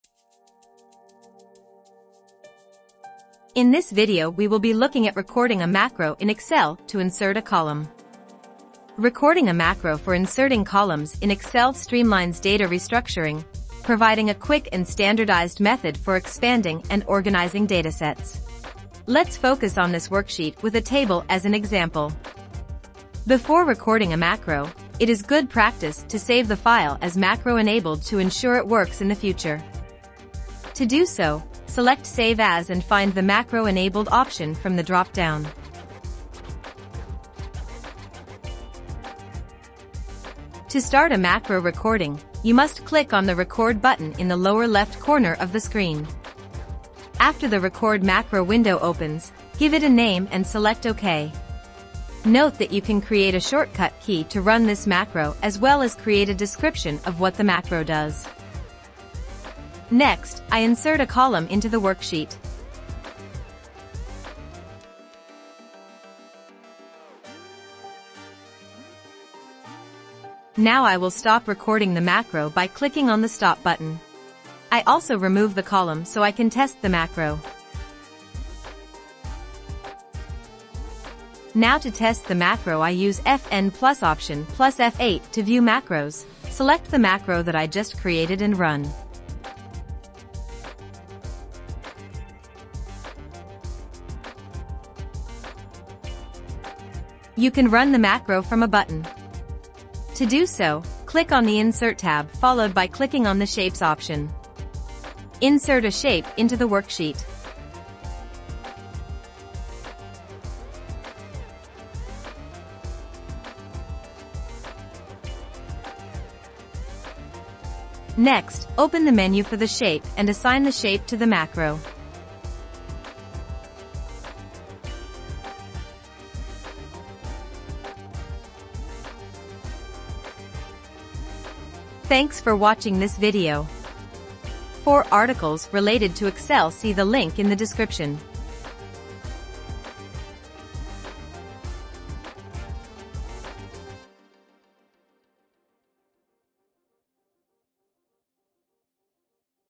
Recording a Macro to Insert a Column in Excel
SUMMARY

Here I discuss how to record a macro that will insert a column in an Excel worksheet. Recording macros at the most basic helps to build a foundation for people who want to learn more about Visual Basic Applications for Excel.

CHAPTERS
00:29 Save Workbook as macro-enabled

My Gear:
Mac Book Pro
LUMIX S5 Camera